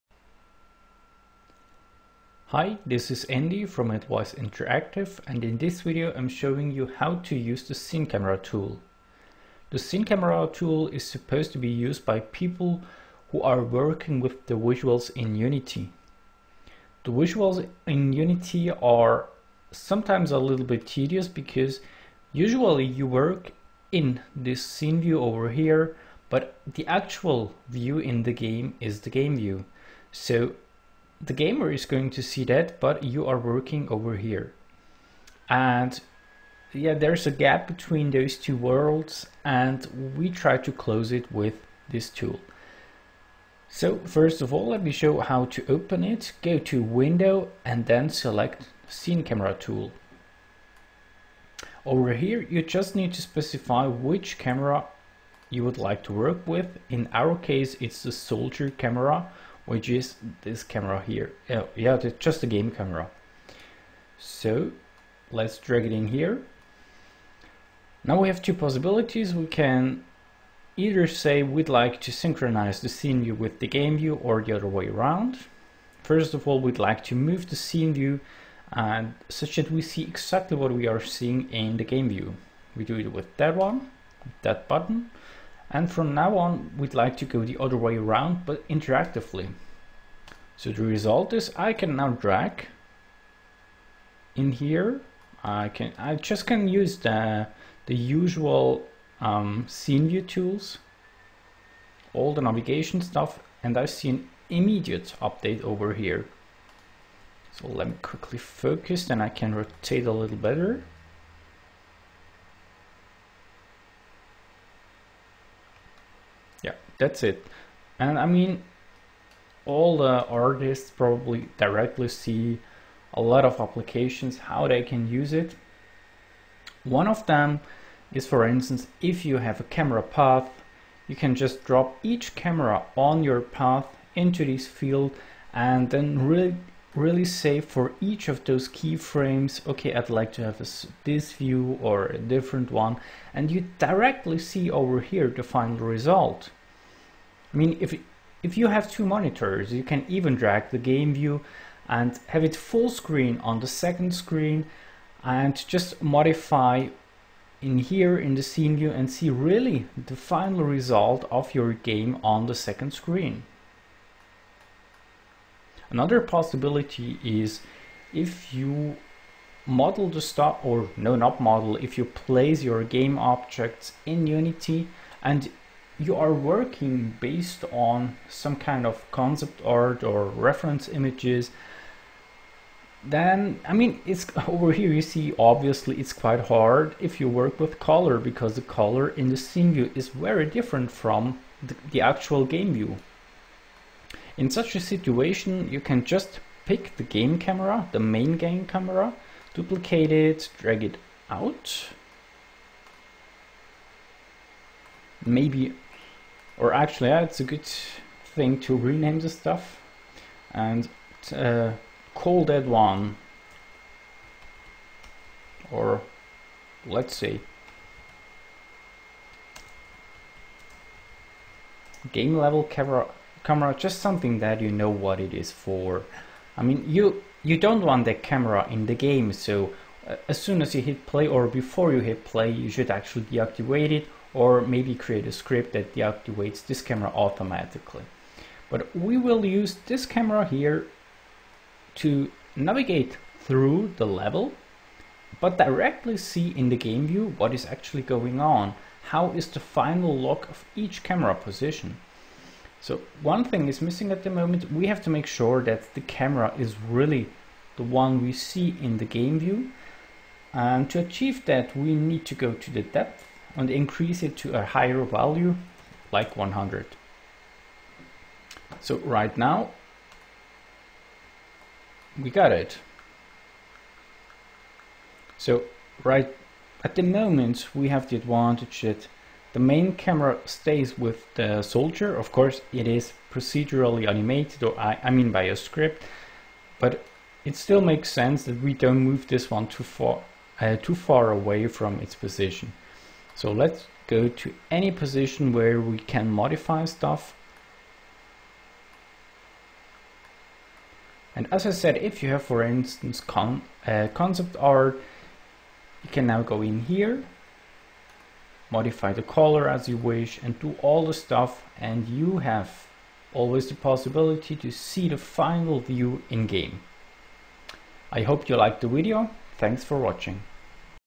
Scene Camera Tool
Align any camera in Unity with the active scene view. Using the scene view navigation in combination with the automatic camera synchronization simplifies and accelerates your workflows.

Camera Positioning
Place the camera with the comfortable scene view navigation.

Level Editing
With the scene and game view side by side, it becomes possible to edit the level and to see the in game result directly.